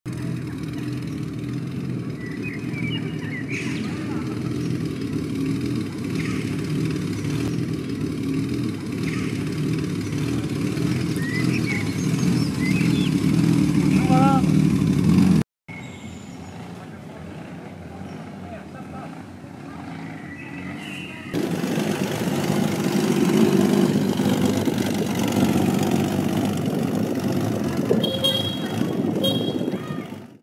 Balade en side-car au Centre gériatrique des Capucins
Les résidents du Centre gériatrique des Capucins ont la chance de se voir proposer des balade en side-car depuis le printemps 2023 et pour tout le temps de l'été, à raison d'une fois par mois. Cette animation est proposée bénévolement par Christian PETITBON, sur une idée de Valérie, aide-soignante de la résidence Goélo 1.
Les résidents en redemande !